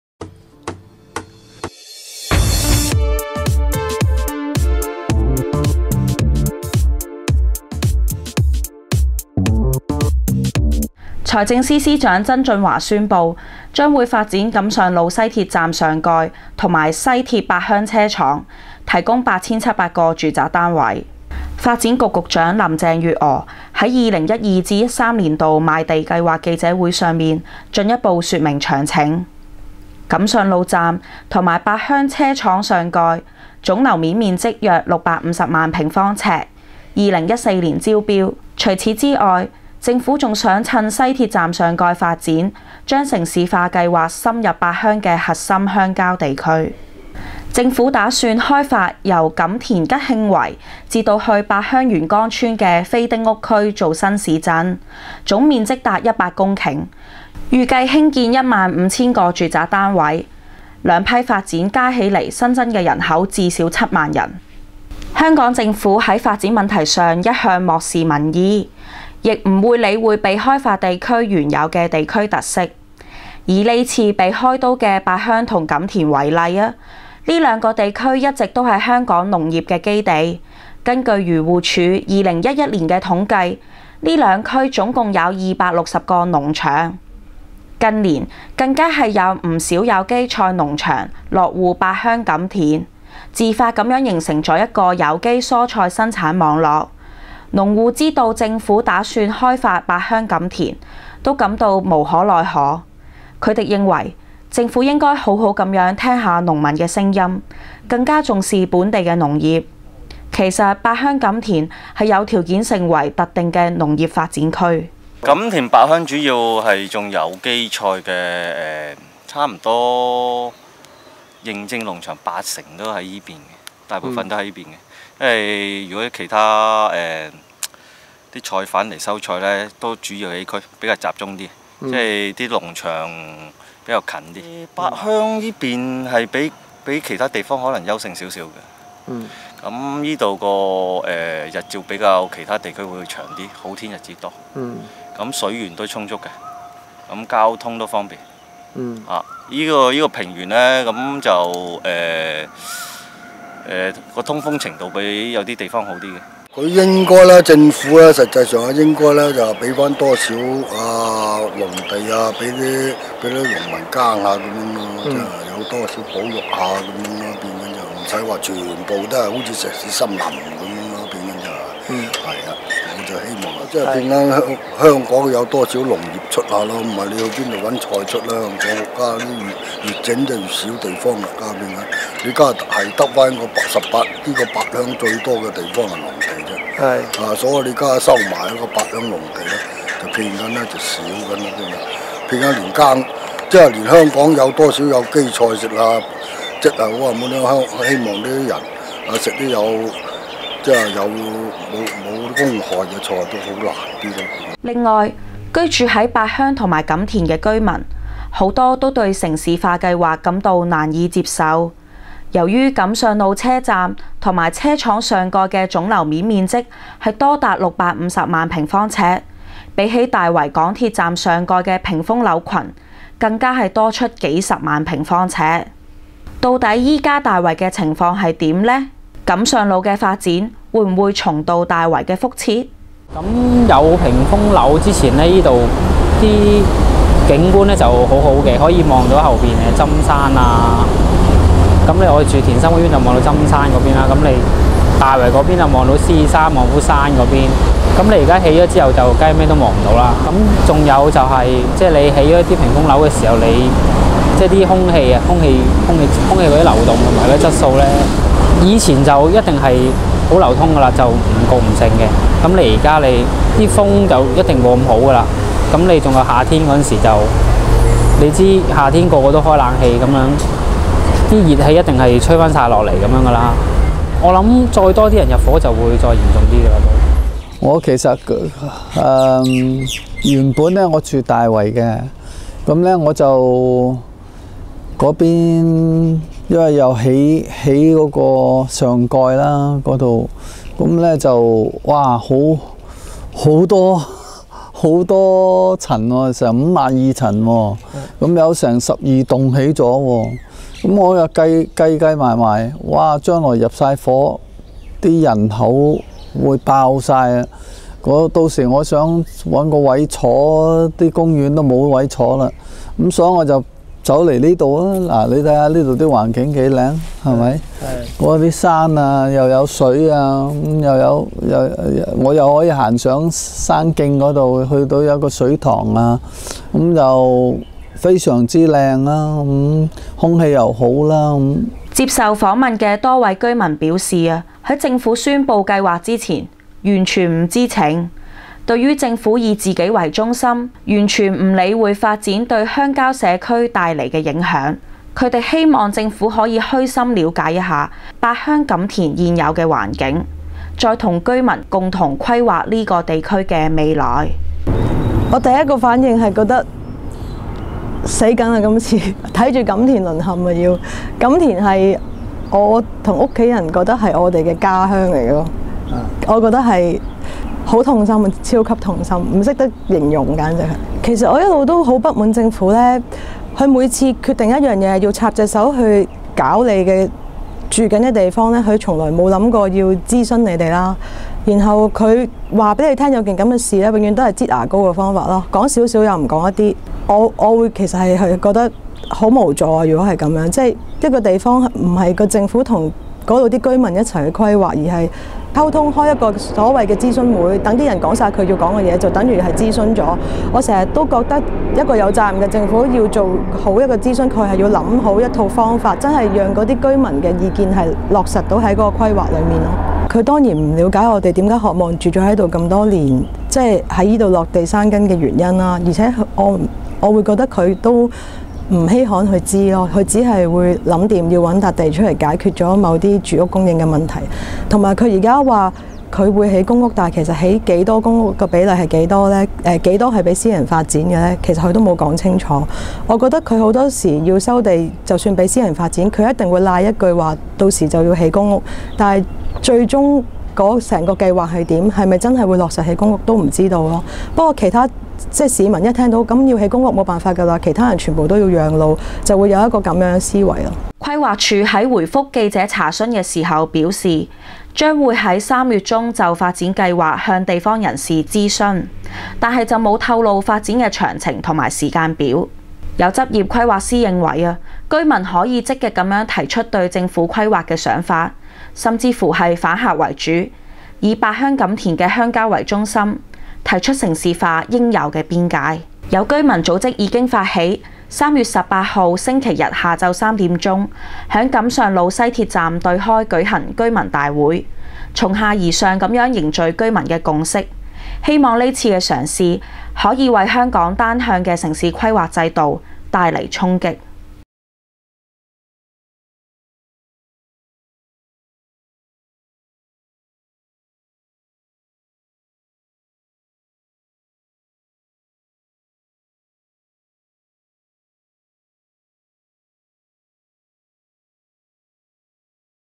獨立媒體影像報道：屏風樓殺到錦上路 居民希望留住鄉郊
在二月初公布的財政預算案，住宅供應是其中一個焦點。財政司曾俊華宣布，將會發展錦上路西鐵站上蓋及西鐵八鄉車廠提供8700個住宅單位。錦上路站和八鄉車廠上蓋，總樓面面積約650萬平方尺，比起大圍港鐵站上蓋的屏風樓群更多出幾十萬平方尺。除此之外，政府還想趁西鐵站上蓋發展，將城市化計劃深入八鄉的核心鄉郊地區，為該區帶來翻天覆地的轉變。